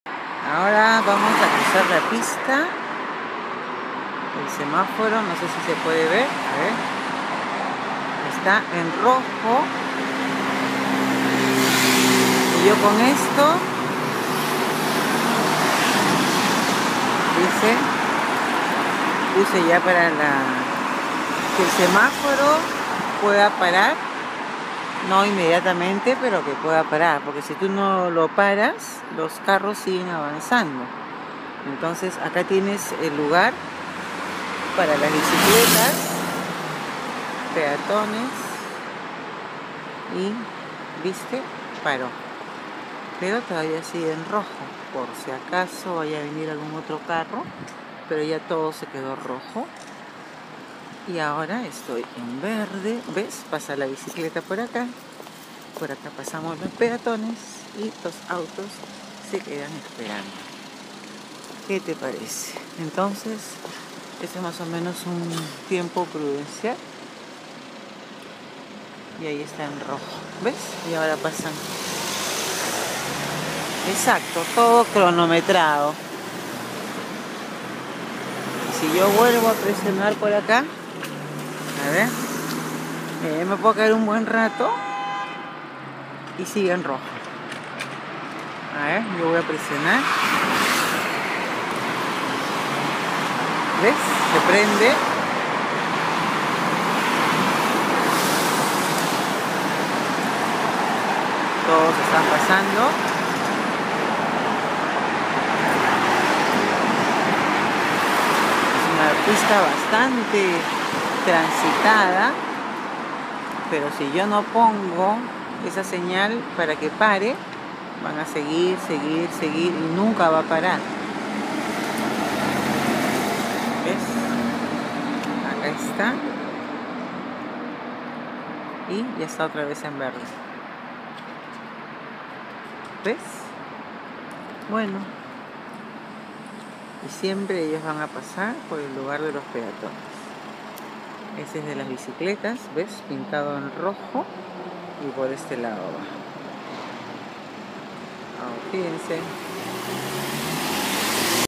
El semáforo, tu lo manejas
En este lugar tu presionas el semáforo para poder pasar, hay mucho orden y se respeta mucho es casi imposible que alguien se lo pase en rojo